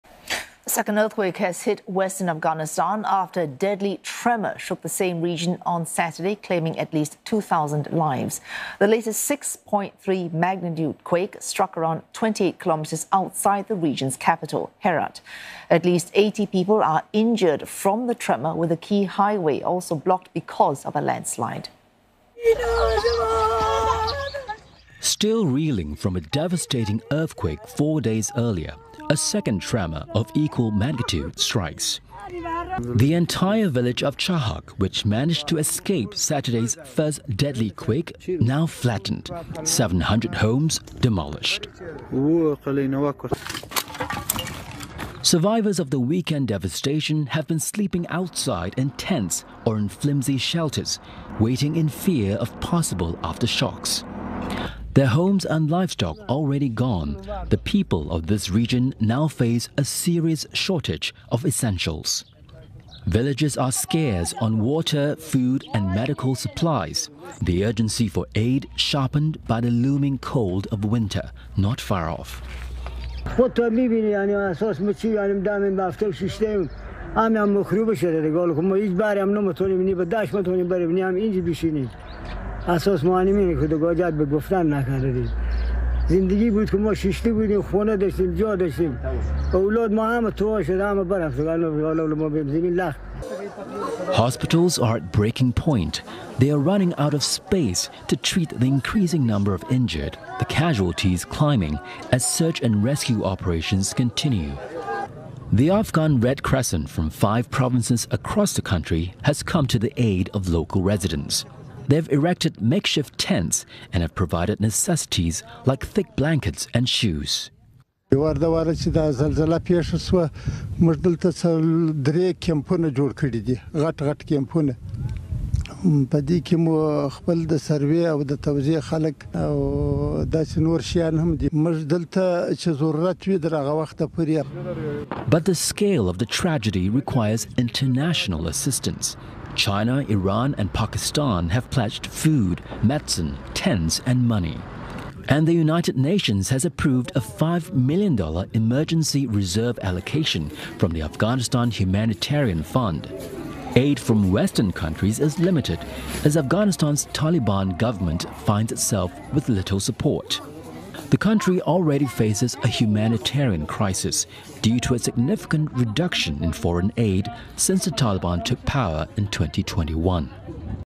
Second earthquake hits western Afghanistan
A second earthquake has hit western Afghanistan, after a deadly tremor shook the same region on Oct 7, claiming at least 2,000 lives. The latest 6.3-magnitude quake struck around 28 kilometres outside the region's capital, Herat. At least 80 people have been injured from the tremor, with a key highway also blocked because of a landslide.